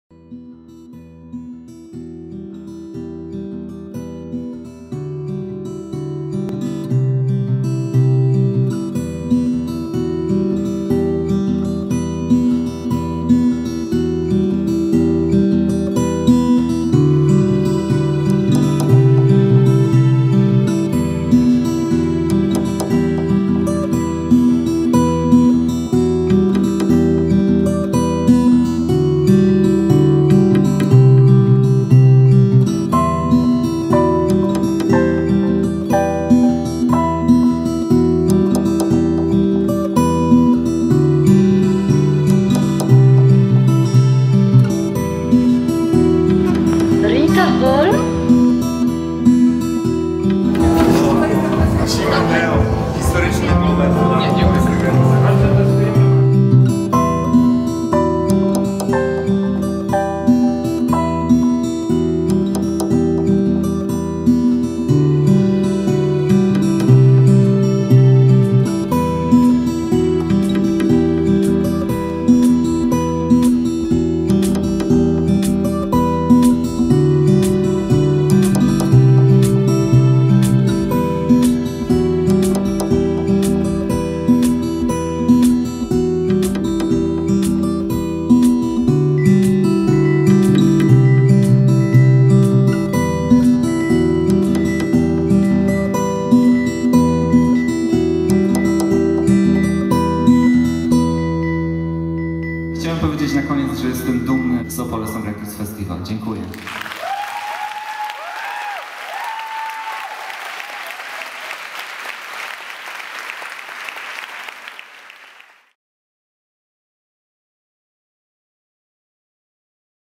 Opole Songwriters Festival 2016
Opole Songwriters Festival 2016 odbyło się w dniach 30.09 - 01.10 w Teatrze im. J. Kochanowskiego w Opolu.

Realizacja: Katarzyna Gierszewska-Widota
Dźwięk: RITAHOLM